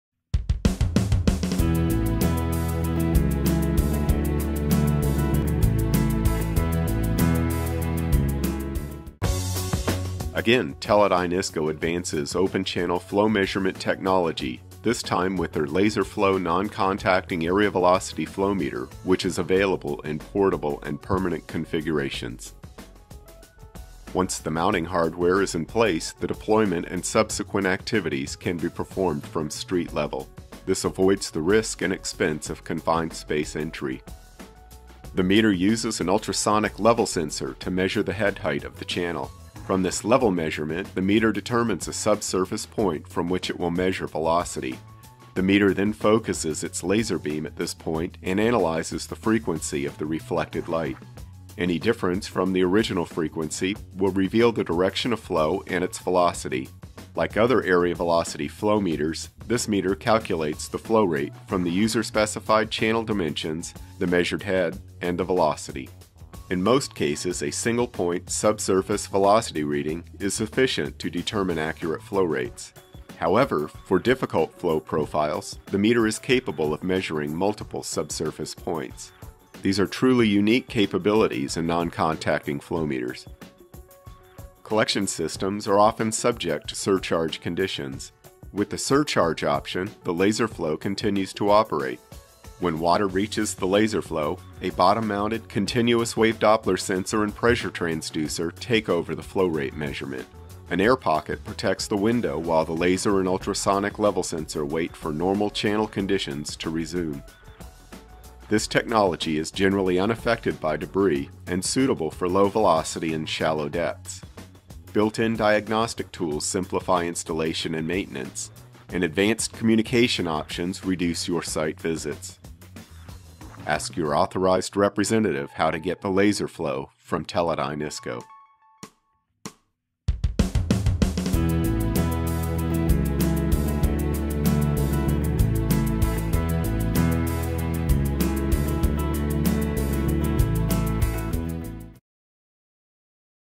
Open Channel Flow Measurement With Laser and Ultrasonics
The LaserFlow™ velocity sensor remotely measures flow in open channels with non-contact Laser Doppler Velocity technology and non-contact Ultrasonic Level technology. The sensor uses advanced technology to measure velocity with a laser beam at single or multiple points below the surface of the wastewater stream.